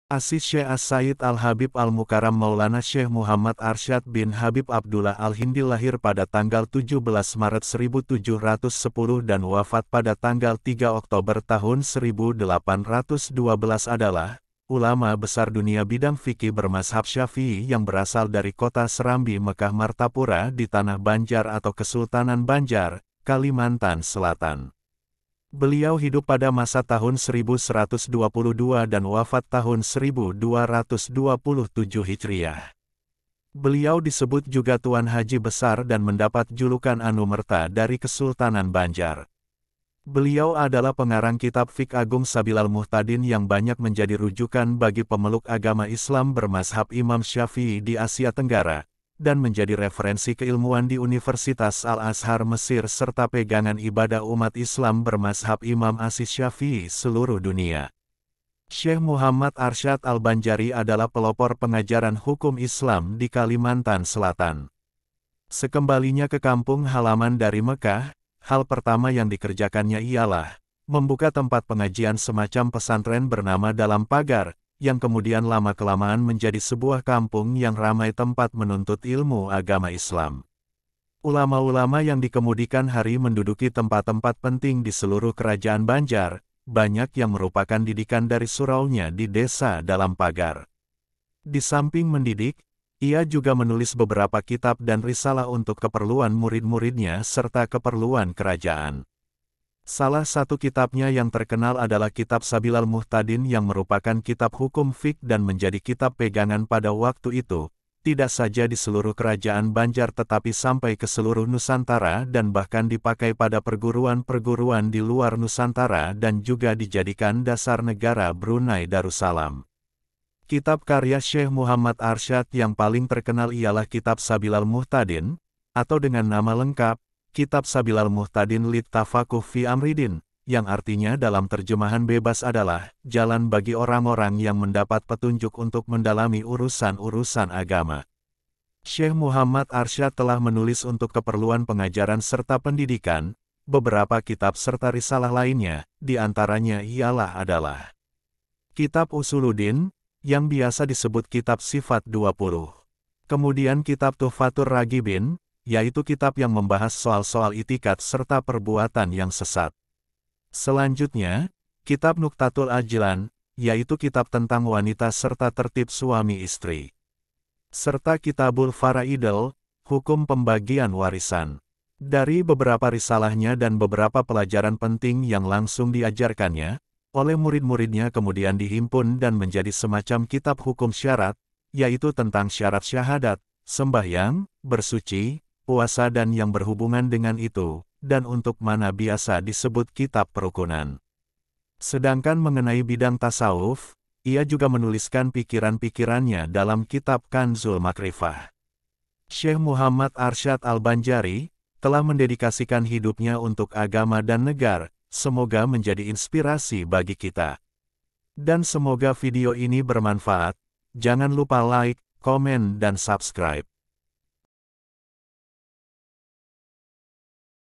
Syeikh Muhammad Arsyad Al-Banjari Ulama Nusantara yang mendunia
video ini menampilkan profil dari ulama kharismatik besar dari Nusantara tepatnya dari kerajaan Al Banjar yang ilmunya sangat luas. beliau adalah Syeikh As Sayyid Al Habib Muhammad Arsyad Al-Banjari.